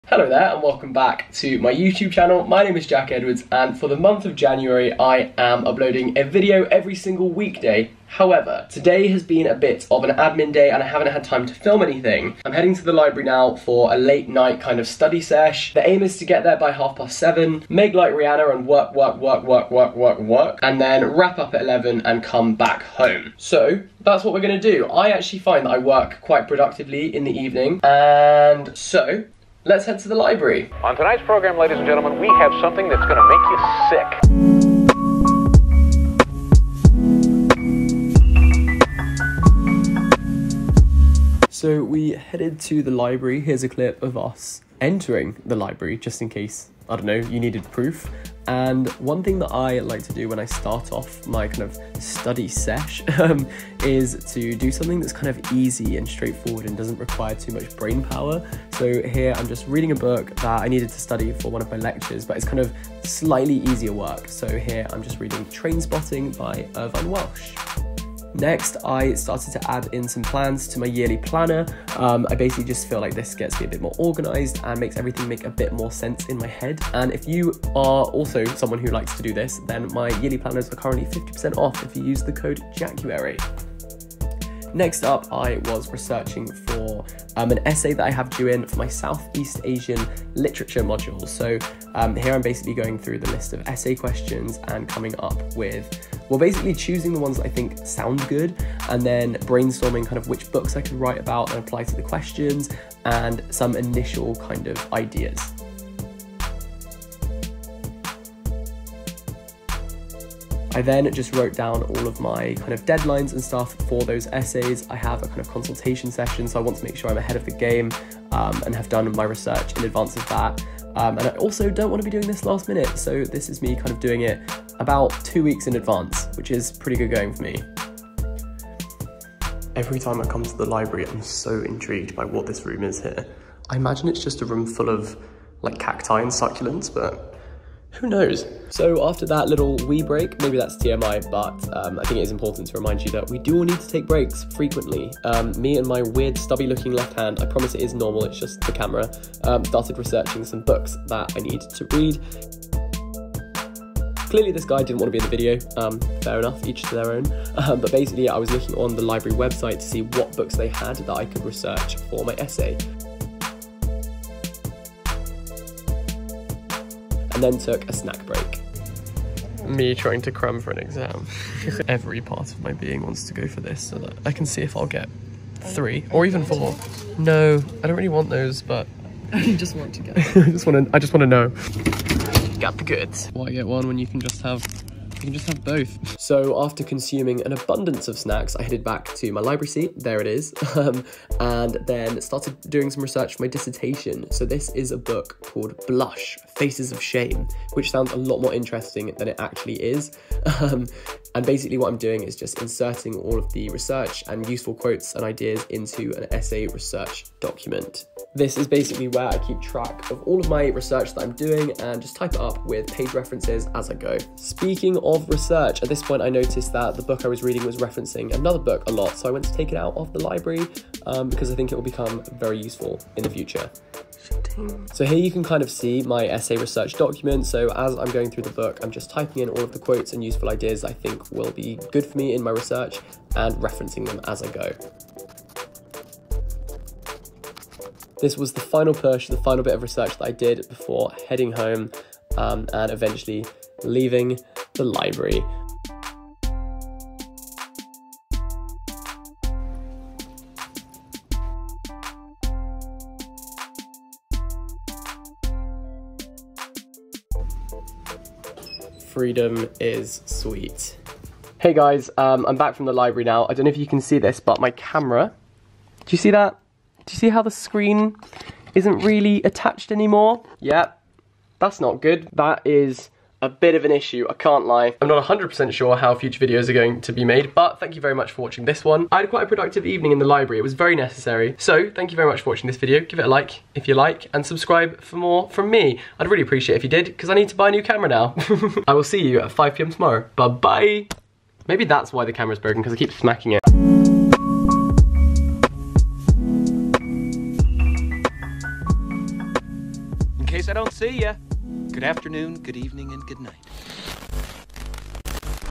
a late night library trip (study with me)
i had a very productive trip to the library late last night, so decided to vlog it and hopefully give you some motivation for how you can utilise your evenings :)